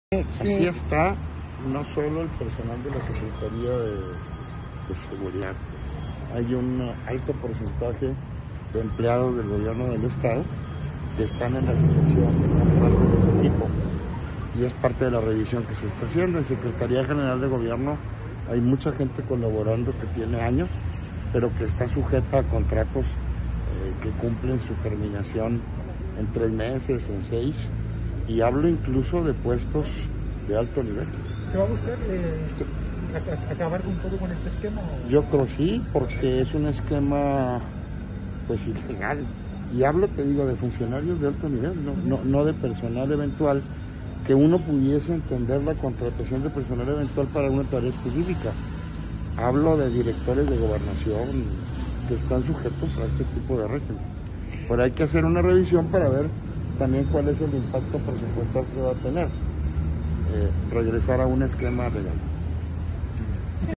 Hay también funcionarios de alto nivel con contratos eventuales: Jáuregui
Chihuahua.- El Secretario General de Gobierno, César Jáuregui Moreno, comentó que al interior del Gobierno se cuenta con un alto porcentaje de trabajadores con contratos eventuales, incluso, mismos funcionarios de alto nivel, no únicamente en la Secretaría de Seguridad Pública Estatal.

En entrevista en las inmediaciones del Palacio de Gobierno, Jáuregui Moreno señaló que en relación a los contratos de los trabajadores del estado, dicha practica de entregas de contratos por 3 a 6 meses se ha registrado también en puestos de alto nivel.

‘’Hablo de directores de Gobernación que están sujetos a este tipo de régimen, pero hay que hacer una revisión para ver el impacto presupuestal que pudiera tener’’,  dijo.

Gilberto Loya, Secretario de la SSPE, informó ayer por la tarde que, le causó impresión el contar en la institución con policías que cuentan con contratos eventuales.